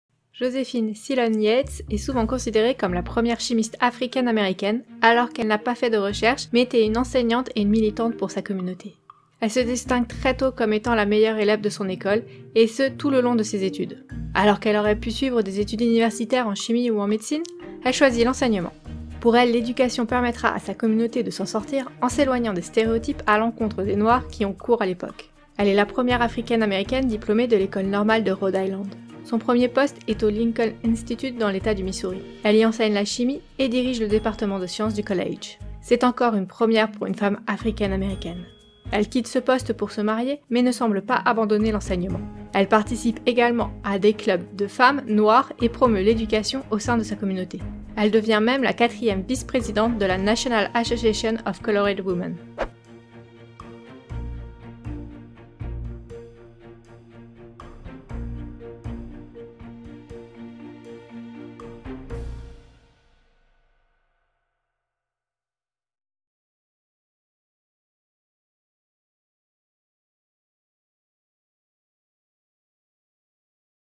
Josephine Silone Yates - Enseigner pour émanciper - Micro-bio 25
Josephine Silone Yates (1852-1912) aurait pu être la première chimiste africaine américaine mais elle a préféré enseigner la chimie (entre autres) pour aider les membres de sa communauté à obtenir un meilleur statut. Elle est également la première africaine américaine à être directrice d’un département de science d’un établissement supérieur. Et elle a écrit de nombreux articles et livres tant militants que de poésie.

Bibliographie :
Jeannette E. Brown, African American Women Chemists, New York, Oxford University Press, 2012, 252 p.
Gary R. Kremer et Cindy M. Mackey, « 'Yours for the Race': The Life and Work of Josephine Silone Yates », Missouri Historical Review, vol. 90, no 2,‎ 1996, p. 199–215

Musique par Jingle Punks

Images :
Josephine Silone Yates vers 1902, extrait de Twentieth century Negro literature; or, A cyclopedia of thought on the vital topics relating to the American Negro par Daniel Wallace Culp - Domaine Public
Josephine A. Silone Yates vers 1885, Library of Congress - Domaine Public
Bannière de l’Oklahoma Federation of Colored Women's Clubs (vers 1924), Collection of the Smithsonian National Museum of African American History and Culture - CC0